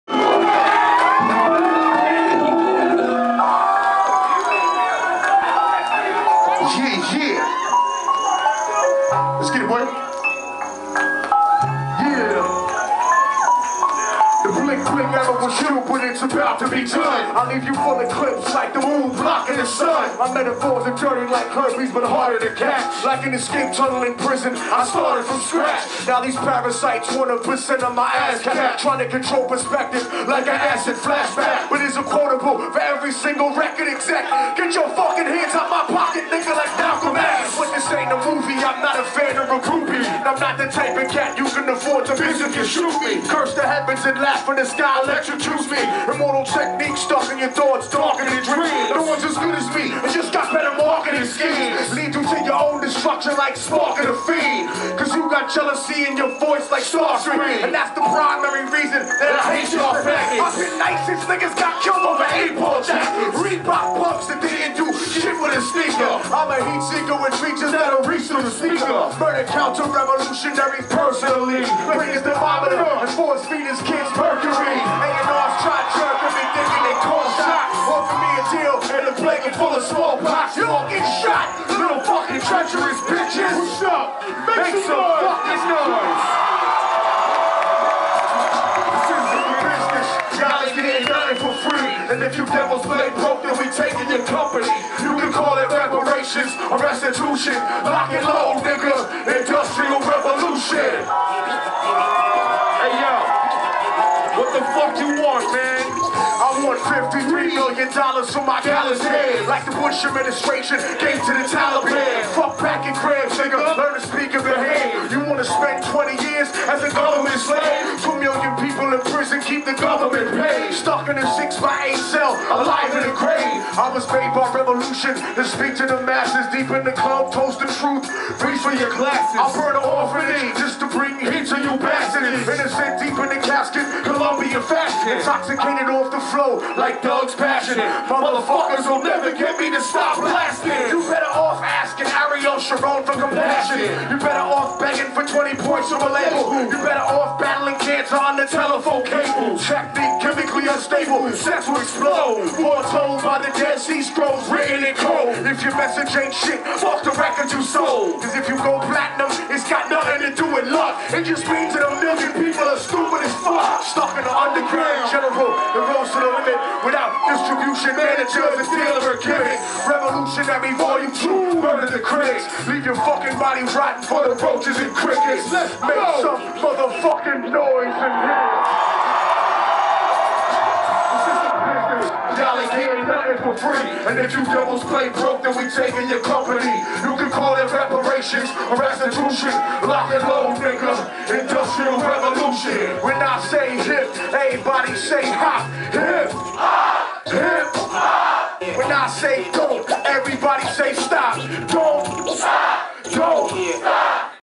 Immortal Technique @ The New Parish
Immortal technique performing at The new Parish, oakland CA on 4/3/11.